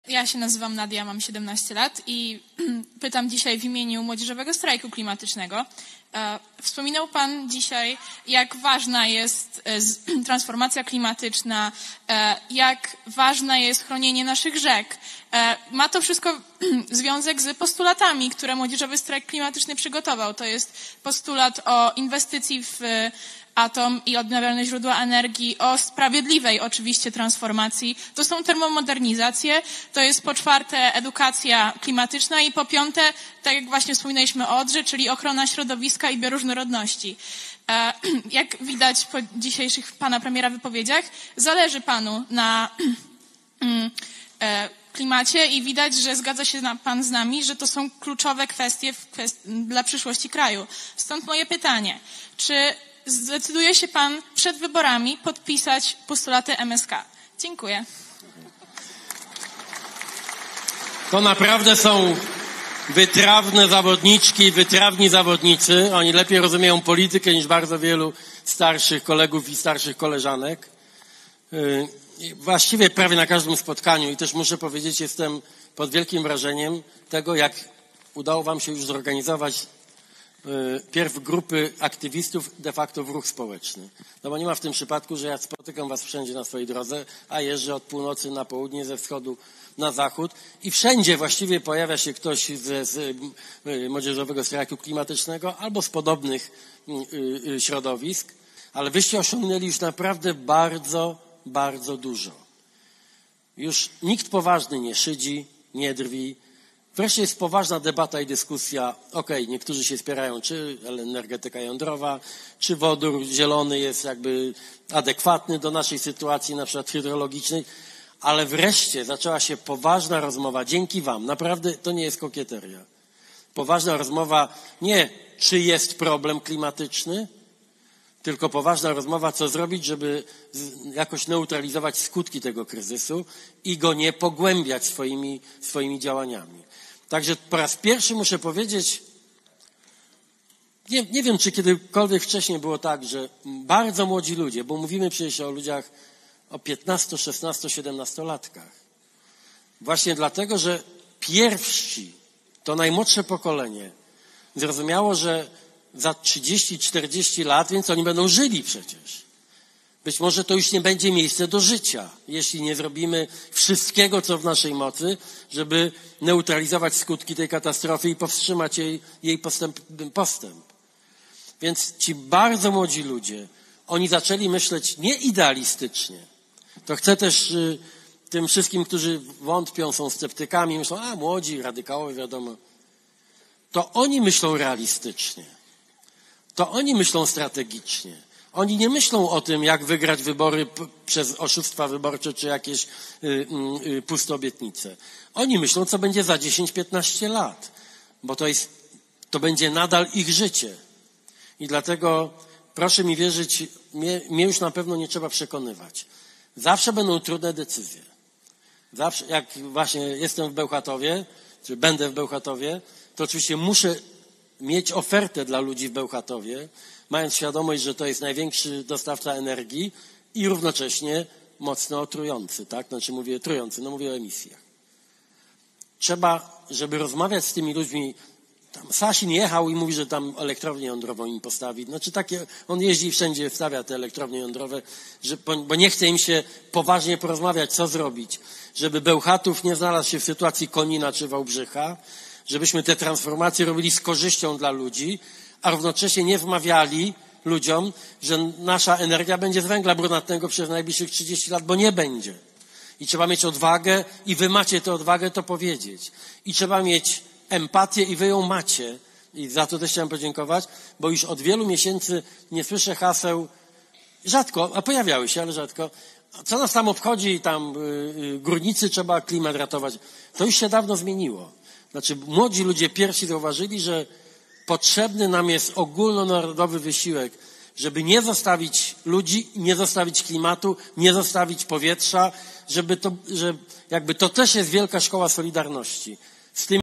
Tusk o aktywistach klimatycznych: Ci bardzo młodzi ludzie myślą realistycznie
Podczas środowego spotkania z liderem Koalicji Obywatelskiej Donaldem Tuskiem na Sisu Arenie w Bydgoszczy ze strony Nadii, 17-letniej aktywistki klimatycznej padło pytanie o poparcie dla postulatów Młodzieżowego Strajku Klimatycznego. Były premier przyznał, że starsi politycy w tym zakresie powinni się uczyć od młodych ludzi.

- To naprawdę są wytrawne zawodniczki i wytrawni zawodnicy, oni lepiej rozumieją politykę, niż wielu starszych kolegów i koleżanek – mówił o aktywności Młodzieżowego Strajku Klimatycznego  lider KO Donald Tusk -  Wyście osiągnęli już naprawdę bardzo dużo – już nikt poważny nie szydzi, nie drwi – wreszcie jest poważna debata, dyskusja. Wreszcie rozpoczęła się poważna rozmowa – dzięki wam – nie czy jest problem klimatyczny, tylko poważna zrobić aby jakoś neutralizować skutki tego kryzysu klimatycznego, a go nie pogłębiać swoimi decyzjami.